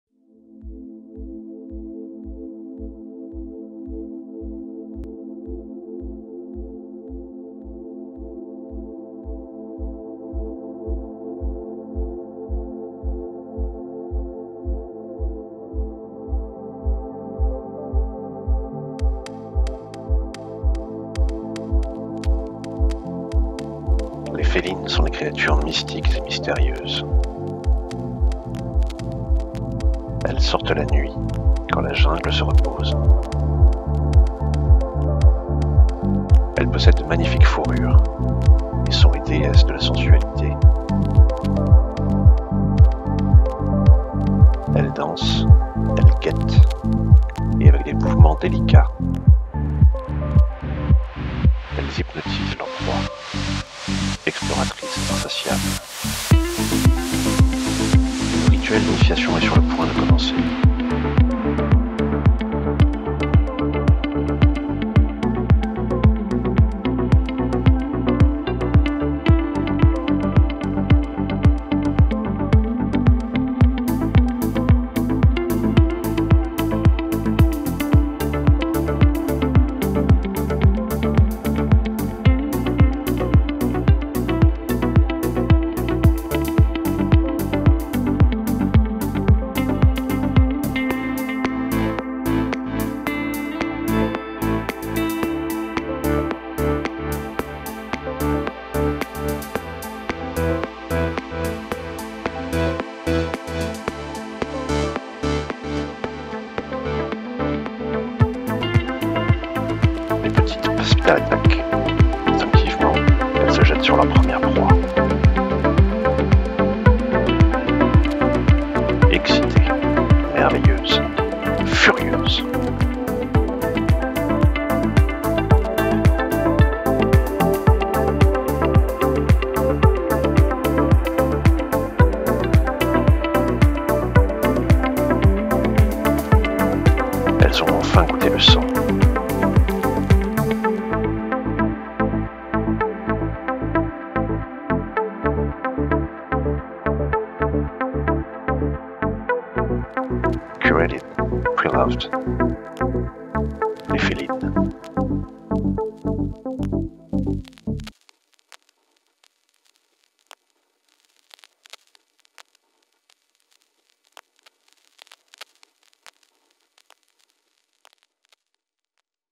Les Félines Fashion Film
Les Félines son una secta de amantes de la moda vintage, curated-preloved second hand. Se reúnen en su guarida para celebrar y transformar la las nuevas cachorras. El rito de iniciación está a punto de empezar.